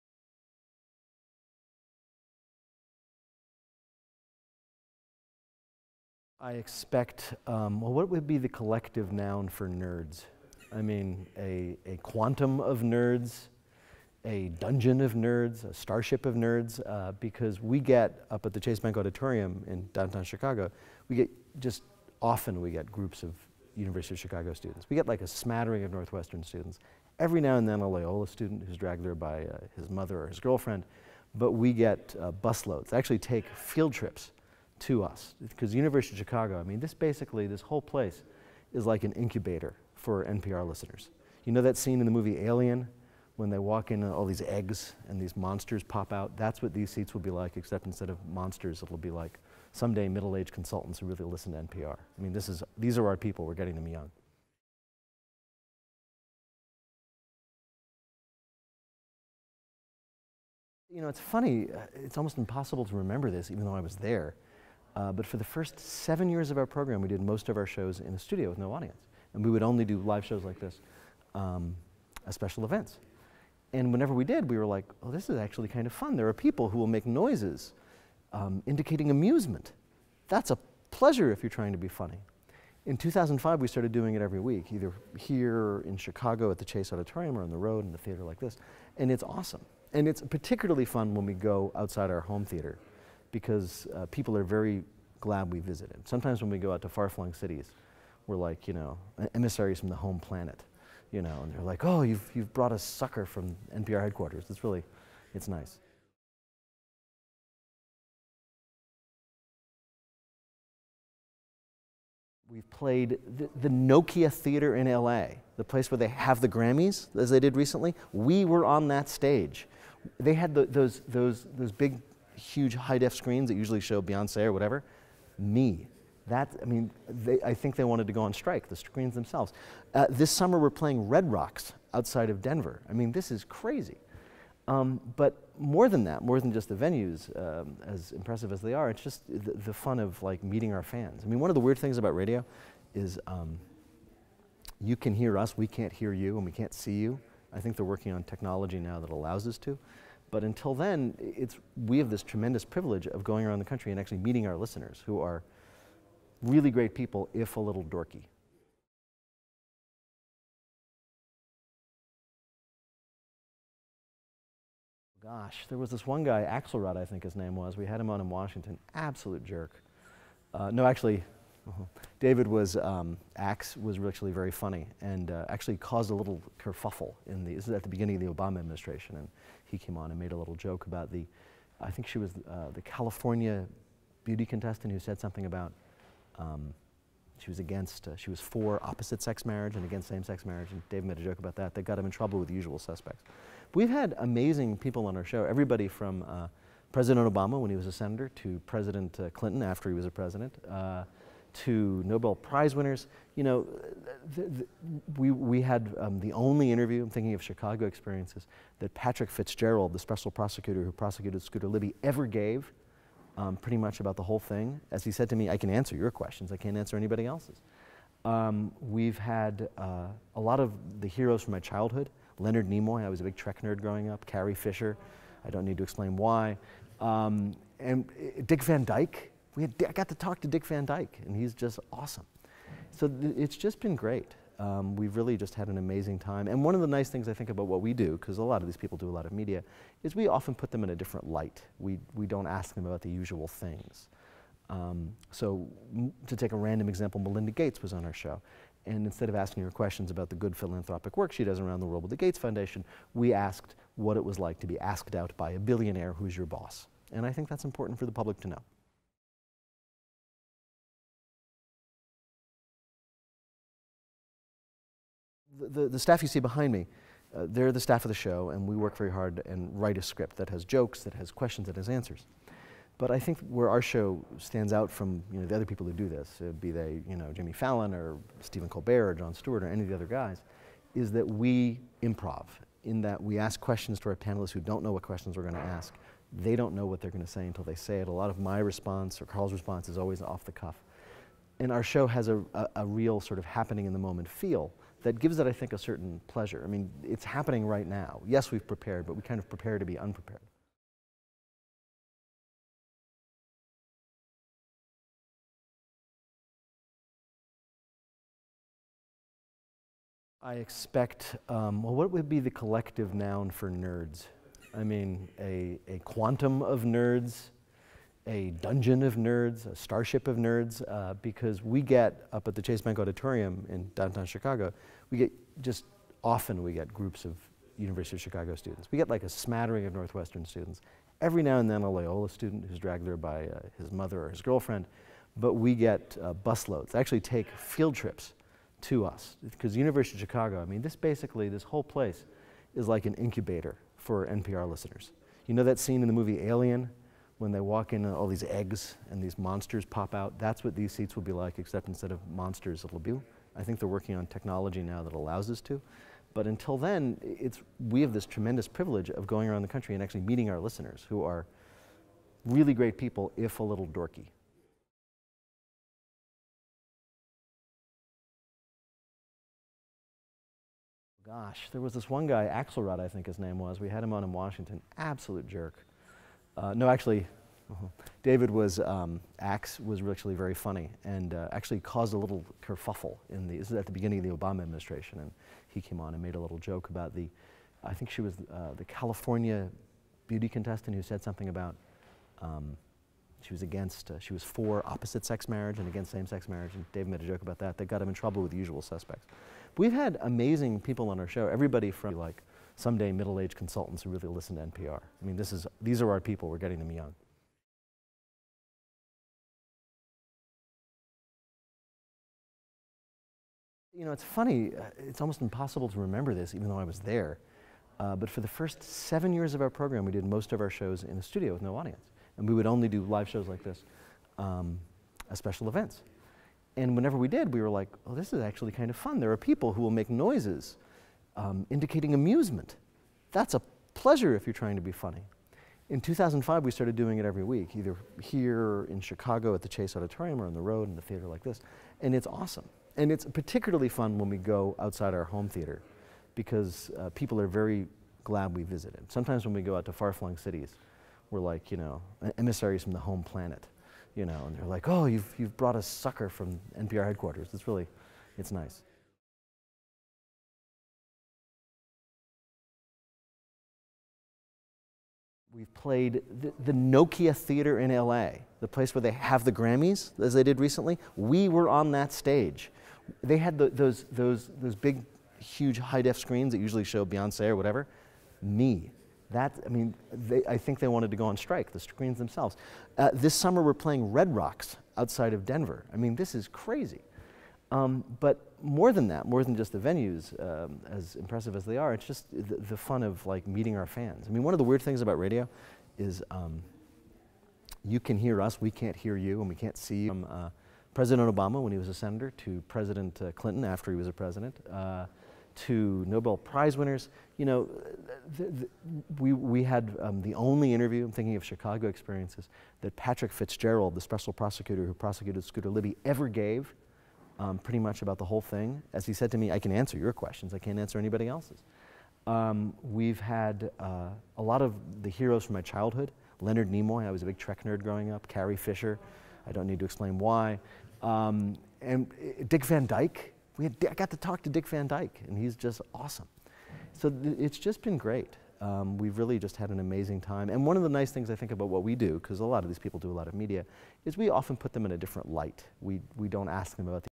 IOPTV: Peter Sagal
Peter Sagal, host of Wait, Wait.Don't Tell Me!, performs some funny one-liners on the November 26th Fear No ART presents The Dinner Party with fellow .